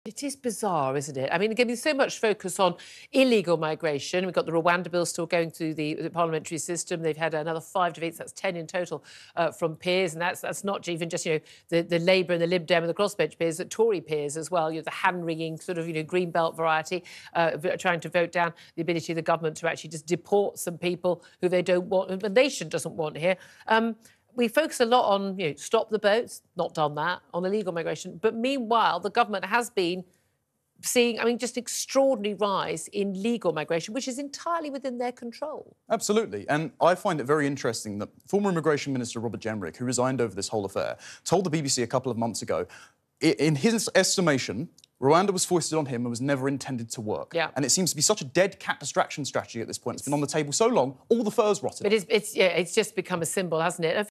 Commentator SLAMS Rwanda Bill As Net Migration Set To Rise By 315,000 Per Year
Experts have warned that net migration in the UK is set to rise by 315,000 per year under the Conservatives, for the next five years.

It comes as the Government’s controversial migration plan, the Rwanda bill, suffered another five defeats in the House of Lords last night.

These statistics, from the Office for Budget Responsibility (OBR), would mean the amount of migrants arriving to the UK each year would be enough to fill a city the size of Doncaster.

TalkTV’s Julia Hartley-Brewer is joined by commentator Connor Tomlinson, and former military intelligence officer Philip Ingram, to discuss the latest in Britain’s continuous migration crisis.

Connor Tomlinson says: “When the money you’re looking at in your pay packet at the end of the month is going down, you don’t exactly feel that diversity is our strength.”